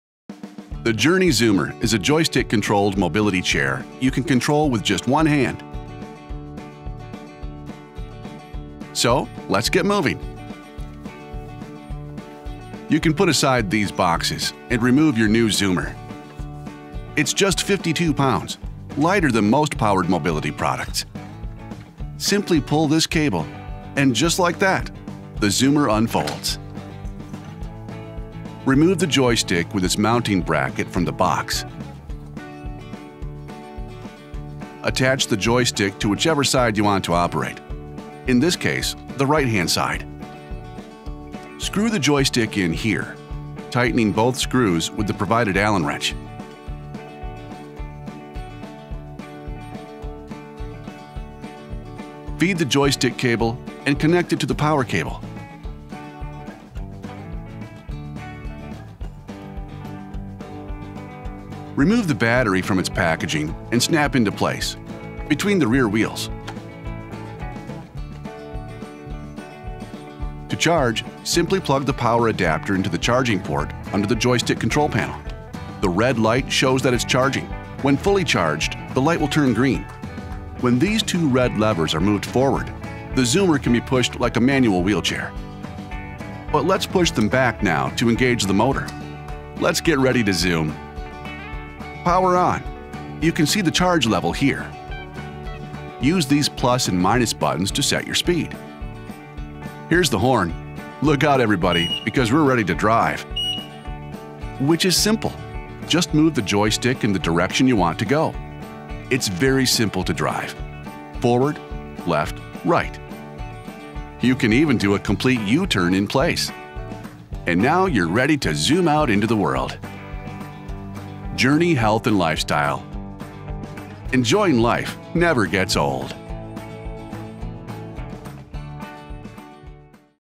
Zoomer Power Folding Chair OneHanded Control Unboxing
Welcome to "The Journey Health Zoomer" unboxing! In this video, we'll be taking a closer look at the Zoomer Power Folding Chair and its remarkable one-handed control feature.

Are you ready to experience the convenience and innovation of the Zoomer Power Folding Chair? Join us as we unbox and explore this fantastic mobility aid that's designed to make your life easier.

In this video, you can expect:

Unboxing of the Zoomer Power Folding Chair.
A detailed demonstration of the one-handed control feature.
Insights into the design and functionality of this incredible mobility solution.
Don't miss out on this exciting unboxing and review! If you're looking for a mobility aid that offers both style and convenience, the Zoomer Power Folding Chair might be just what you need.